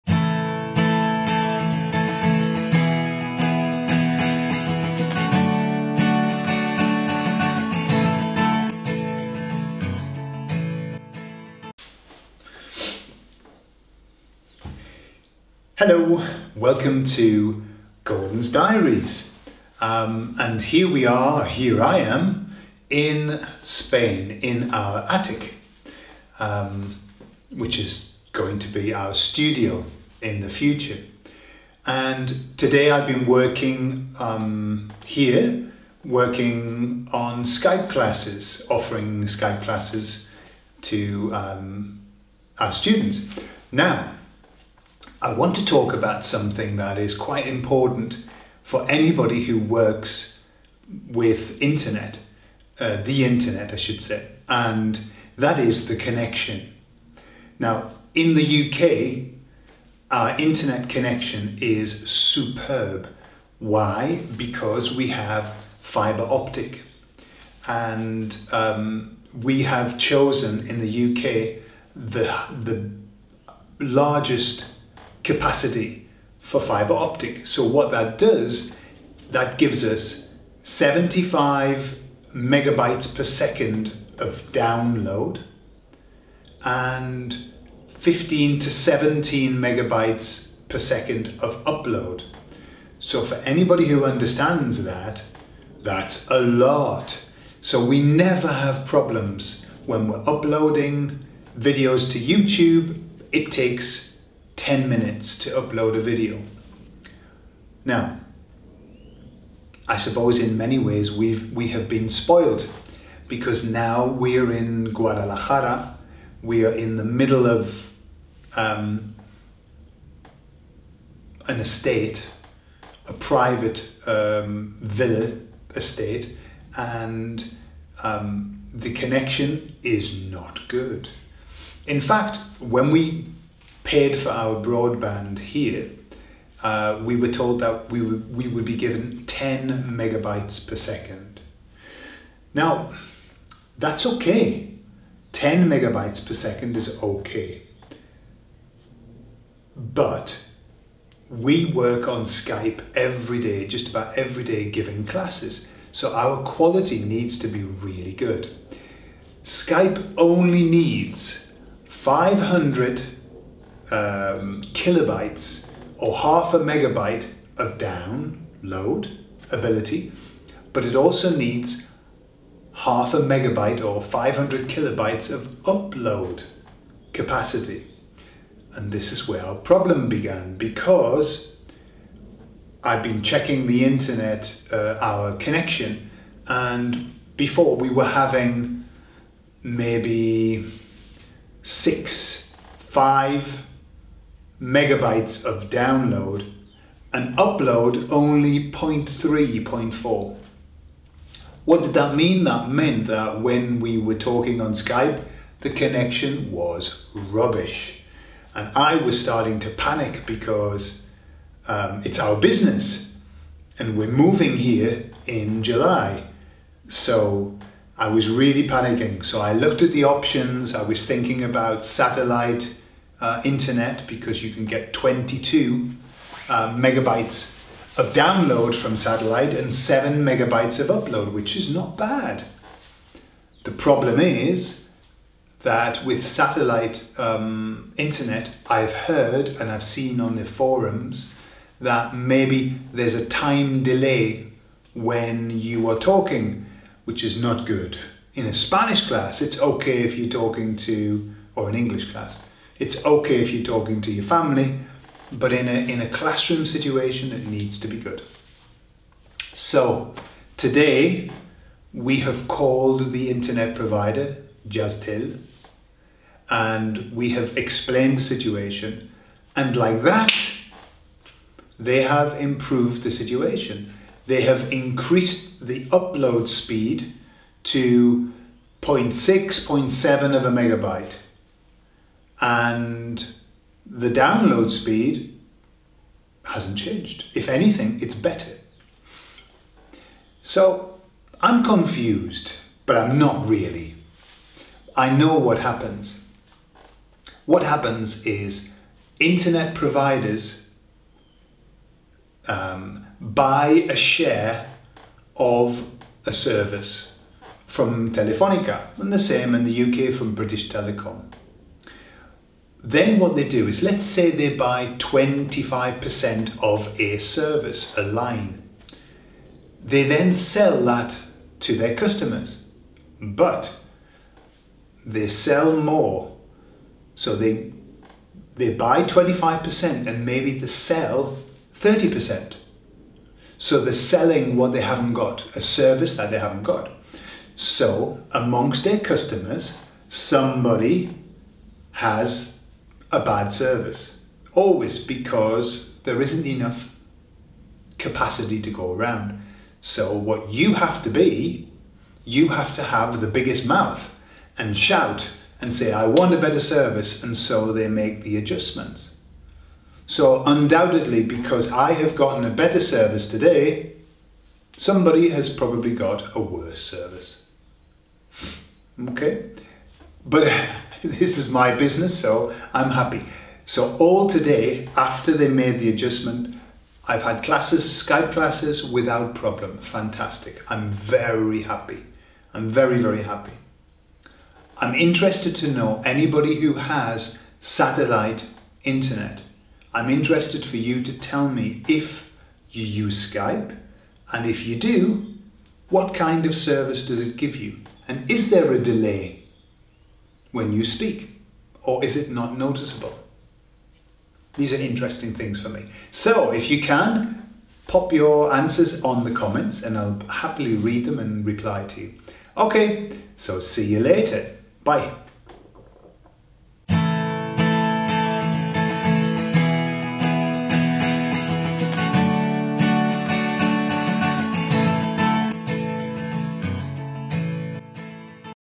9. Gordon's Diaries Internet LightSpeed English
Este es el vídeo número 9 en una serie de diarios sobre cosas inglesas/españolas. Hoy, Gordon habla de Internet y de los problemas con su banda ancha.